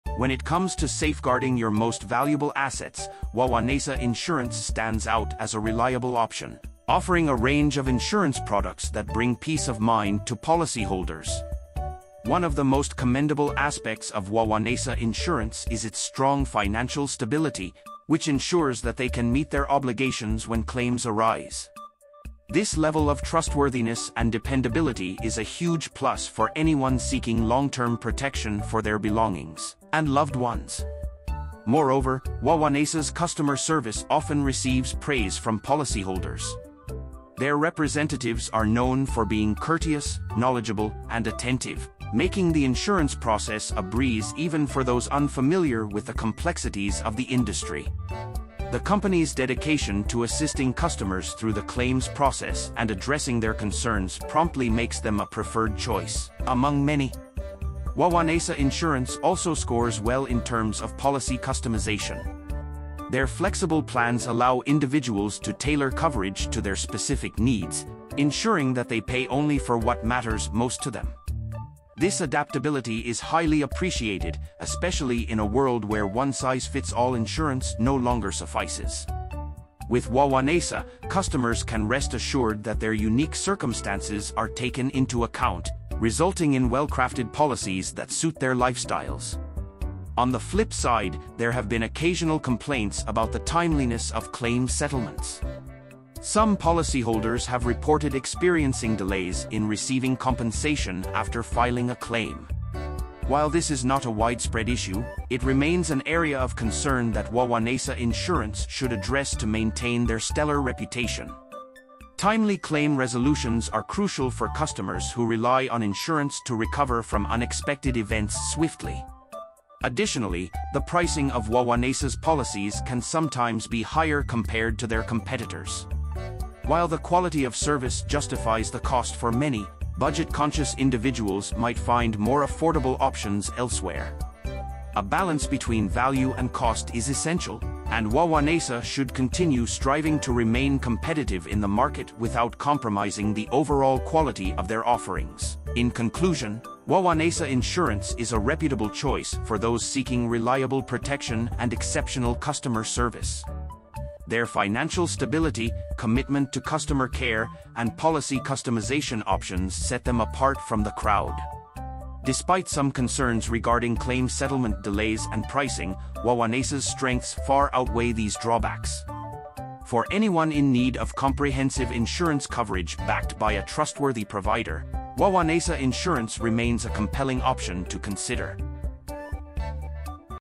🔥 Wawanesa Insurance Review: Protecting Peace of Mind, but with a Few Hiccups
When it comes to safeguarding your future and assets, Wawanesa Insurance stands tall as a beacon of reliability and trust. With a strong track record of financial stability, they instill confidence that your policy will deliver on its promises when you need it most. Wawanesa's commitment to providing top-notch customer service is evident through their dedicated team of professionals, who go the extra mile to ensure your insurance experience is seamless and stress-free.

One of the most appealing aspects of Wawanesa Insurance is their diverse range of coverage options. Whether you're looking for home insurance, auto insurance, or life insurance, Wawanesa has you covered. Their comprehensive policies can be tailored to meet your specific needs, giving you the peace of mind that you're adequately protected from life's uncertainties. This level of flexibility is a huge plus, as it allows you to build a personalized insurance plan that fits perfectly into your lifestyle and priorities.

Wawanesa Insurance has earned a well-deserved reputation for its proactive approach to risk management. Their team of experts is continuously monitoring market trends and assessing potential risks to ensure that their policies evolve with the changing times. By staying ahead of the curve, Wawanesa demonstrates their commitment to keeping you protected in an ever-changing world.

The company's online tools and resources are also worth mentioning. Wawanesa provides an intuitive and user-friendly digital platform, enabling customers to manage their policies, make payments, and access important documents with ease. This level of technological convenience is especially beneficial for those who prefer handling their insurance matters online.

However, like any organization, Wawanesa Insurance isn't without its imperfections. Some customers have expressed frustration with their claim processing times, reporting delays that caused inconvenience during critical situations. While such instances are relatively rare, Wawanesa should continue to focus on streamlining their claims handling processes to enhance customer satisfaction.

Another aspect that warrants attention is the availability of discounts and incentives. While Wawanesa does offer various discounts for safe driving and other factors, they could explore expanding their incentives to attract a broader customer base. Cost-conscious individuals are always on the lookout for ways to save, and a more comprehensive array of discounts could make Wawanesa even more appealing to potential policyholders.

In conclusion, Wawanesa Insurance remains a reliable and customer-centric insurance provider, offering a wide range of coverage options to suit individual needs. Their dedication to financial stability, outstanding customer service, and proactive risk management sets them apart in the industry. As they continue to improve their claims processing efficiency and explore additional cost-saving incentives, Wawanesa Insurance is well-positioned to solidify its position as a go-to choice for comprehensive and trustworthy insurance coverage.